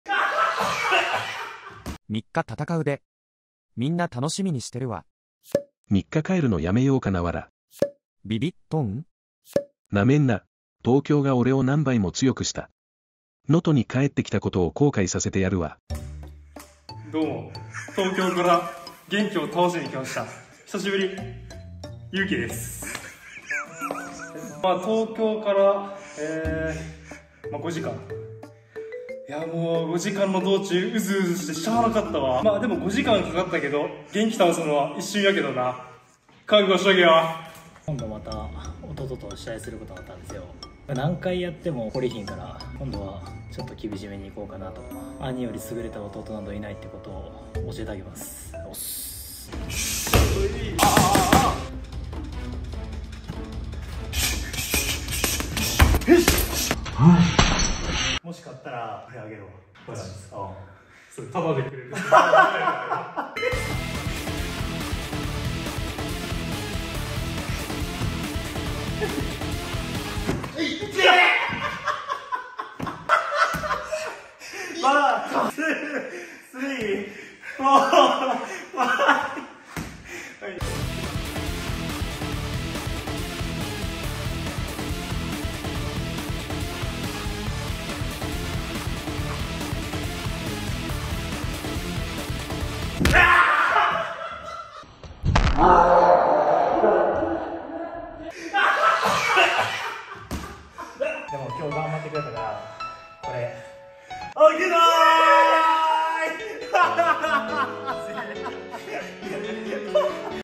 【兄弟喧嘩】空手全日本王者の兄 VS 体重100kgで消防士の弟 殴り合ってみた
▼関連動画